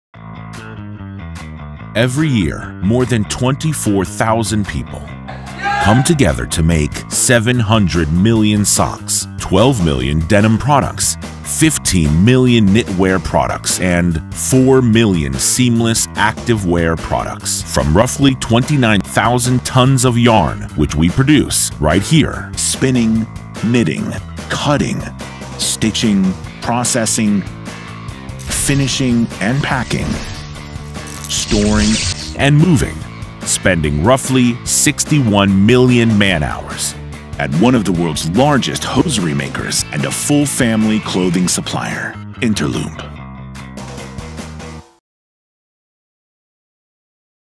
Interloop Corporate Video (short)
Since 1992, Interloop has become known as one of the largest hosiery producers and exporters in the world. Using our manufacturing expertise, we have diversified our offering to seamless activewear, apparel, and denim to become your Full Family Clothing partner of choice. Our efficient and responsible business practices follow a triple bottom approach: people, planet, purpose, ensuring a prosperous future for all and giving you peace of mind knowing business is done right.